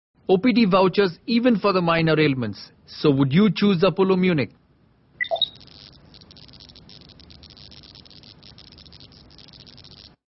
HDFC ERGO Health Episode 9 - Balloon, on the Hot Seat with Sourav Da
Watch him 'light up' as Sourav Ganguly asks him a question.